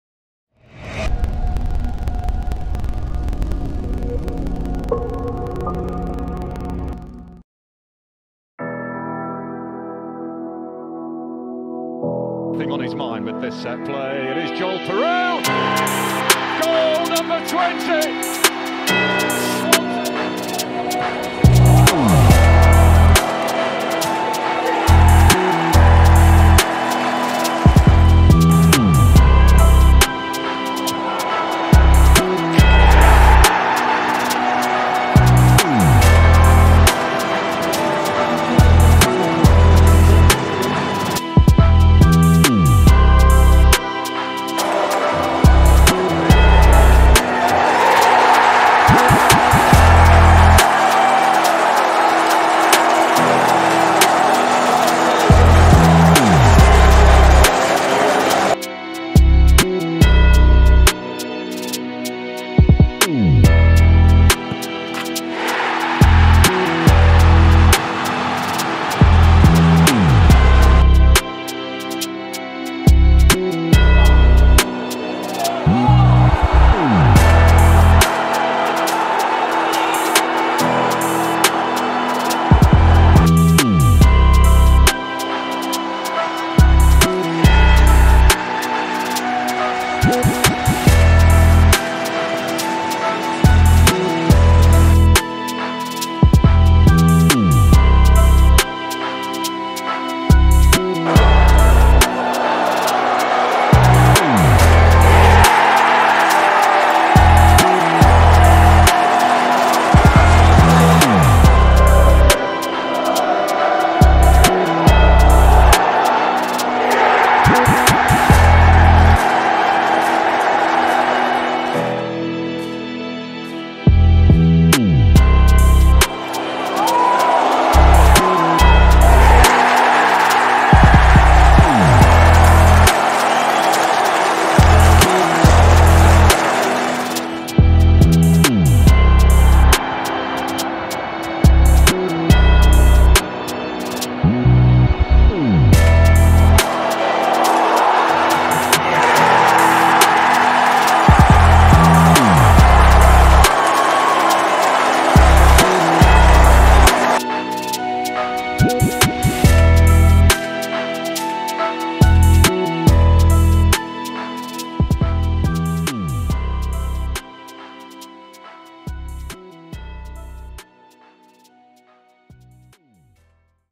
Here's What Joel Piroe Will Bring To Leeds United
Today On Goal Hero we're showcasing Joel Piroe from Leeds United